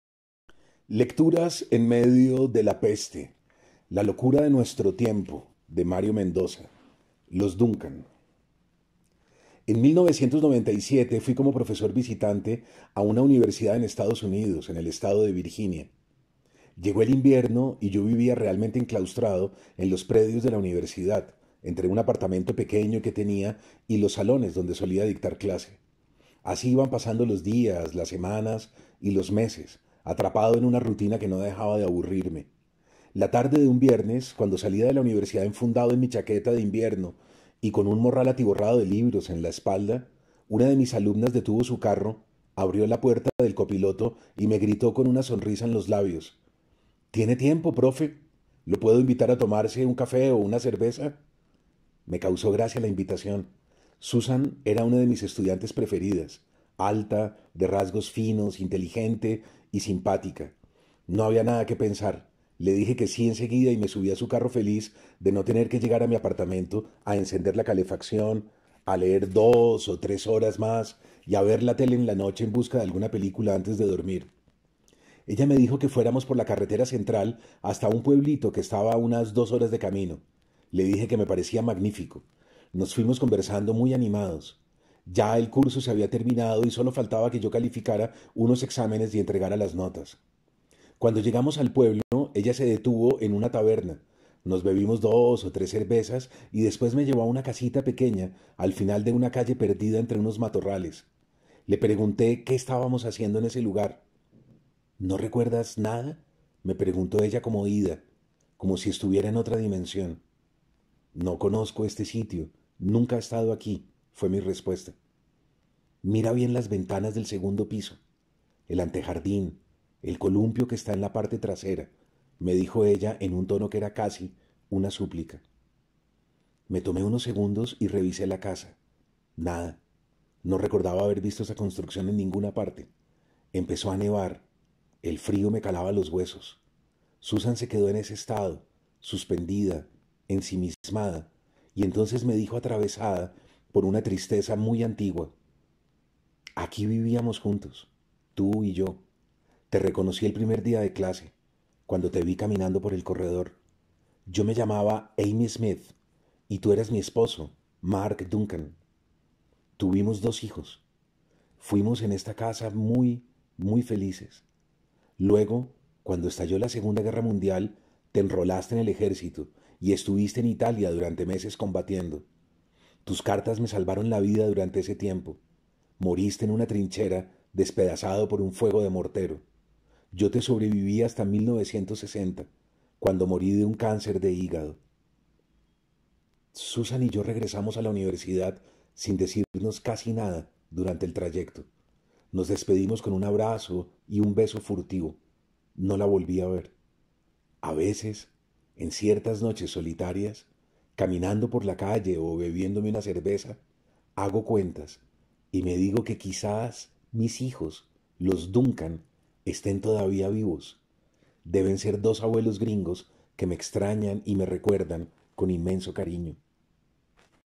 La locura de nuestro tiempo de Mario Mendoza - Los Duncan
🎙 Lecturas en medio de la peste.
En estos tiempos de pandemia le pedimos a Mario Mendoza que leyera fragmentos de sus libros.
Esperamos que lo disfruten.